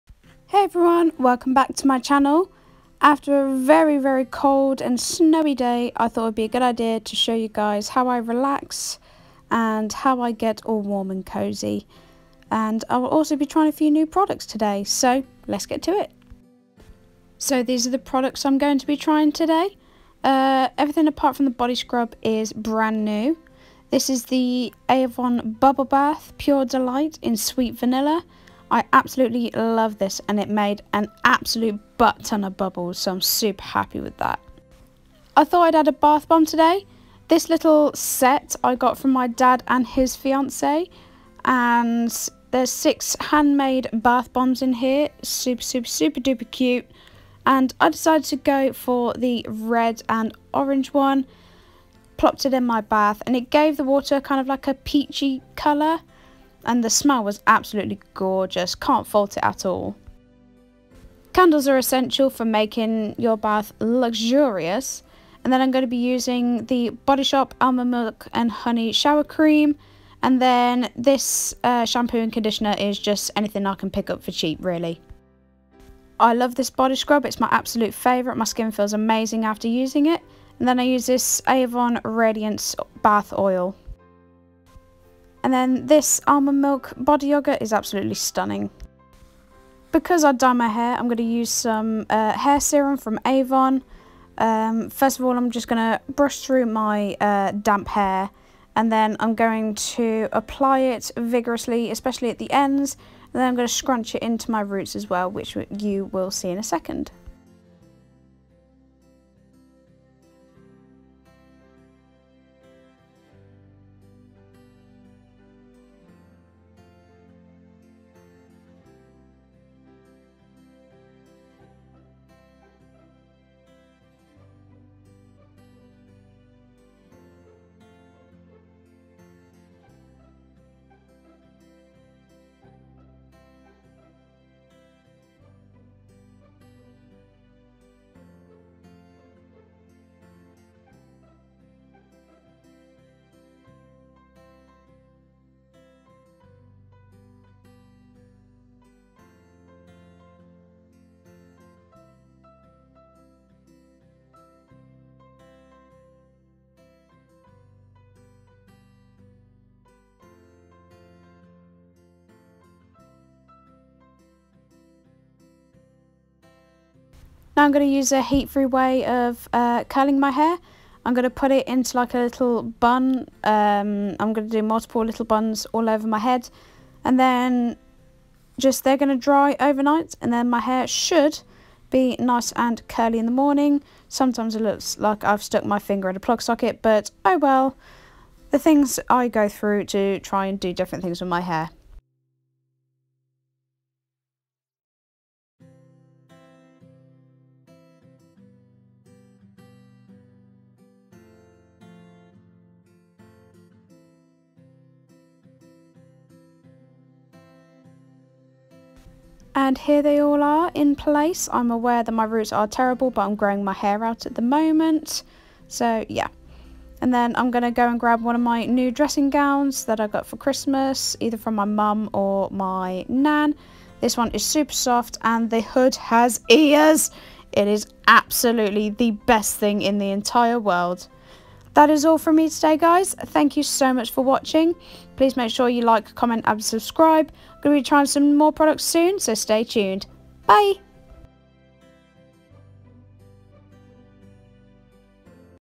Trying New Bath/Beauty Products & Getting Cosy For Winter ❄️ | Chloe May
Hello Everyone!

It's been a while but I wanted to spend some quality time with family during Christmas seeing as I haven't been able to see them during COVID.
I'll put any links to the stuff I used in this video down here if I can find them: